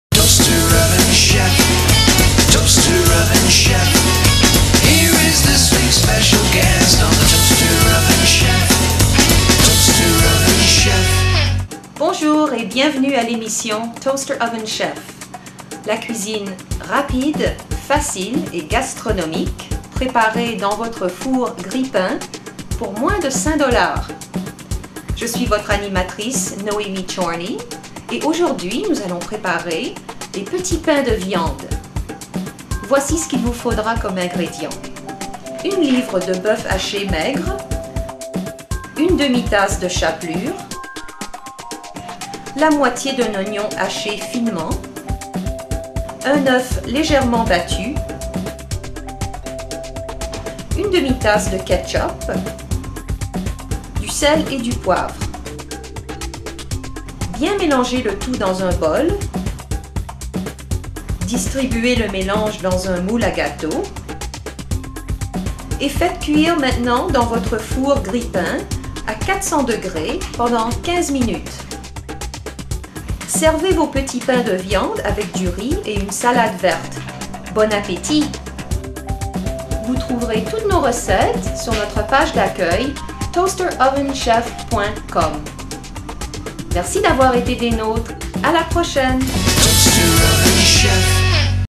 Toaster Oven Chef - Petits Pains de Viande
Recette rapide, facile pour petits pains de viande préparés dans un four-grille pain.

Petits Pain de Viande

1 livre  ou 500g de boeuf haché maigre
½ tasse de chapelure
½ oignon moyen haché finement
1 oeuf légèrement battu
½ tasse de ketchup

Bien mélanger le tout avec vos mains dans un grand bol. Une grosse cuiller en bois fera l'affaire aussi si vous préférez ne pas vous salir les mains.
Répartir des portions égales dans un moule à gâteaux.
Faites cuire à 400°F pour 15 minutes.
Servir avec du riz et une salade verte pour arrondir votre assiette.
Bon appétit!